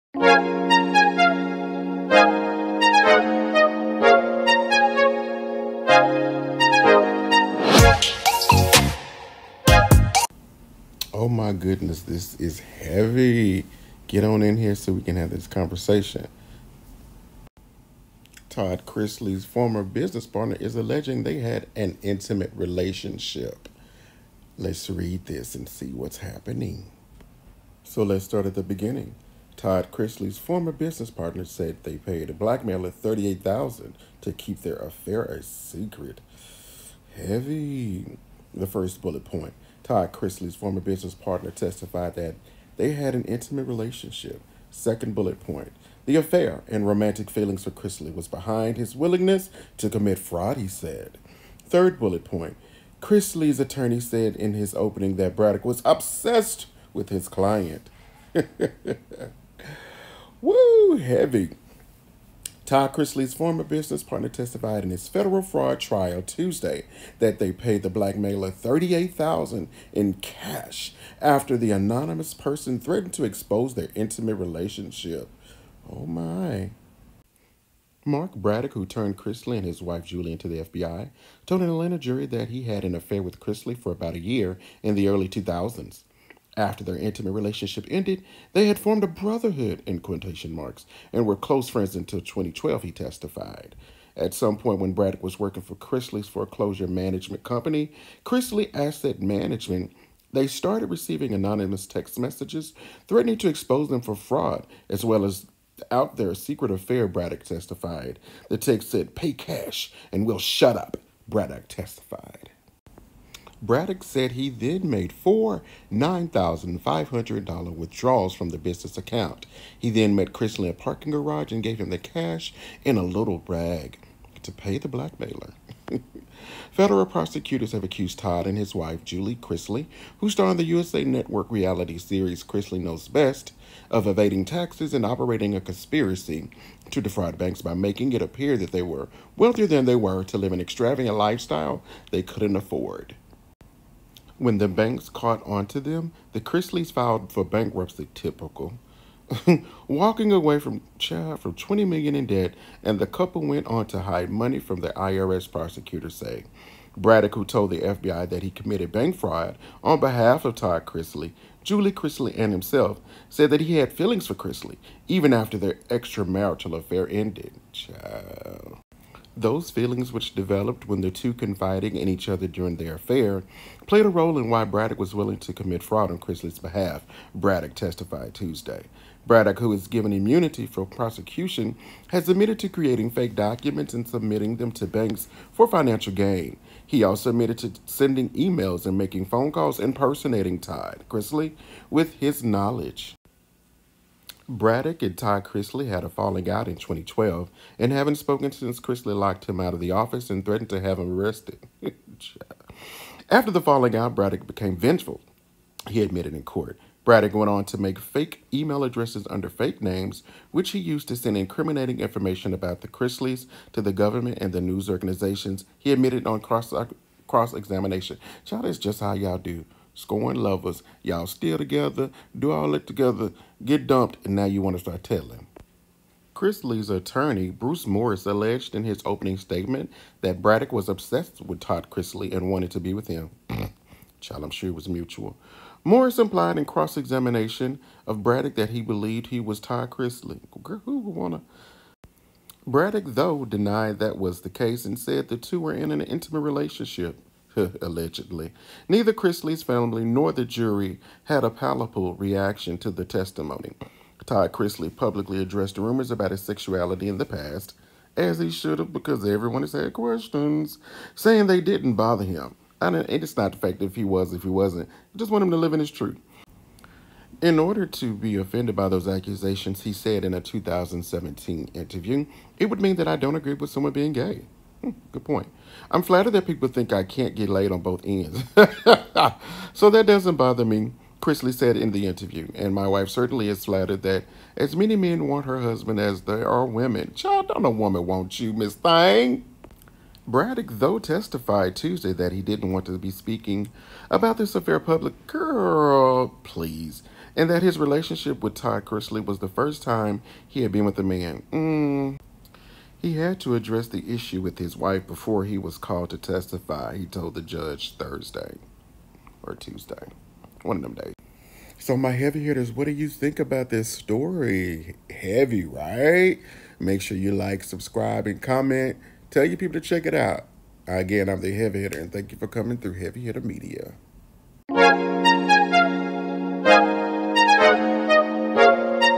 Todd Chrisley of Chrisley Knows Best: ALLEGED FRAUD | GAY AFFAIR | EVADING TAXES This is HEAVY!!!
Media source:
Yahoo news

Wow, this is HEAVY!! ALL IS ALLEGED….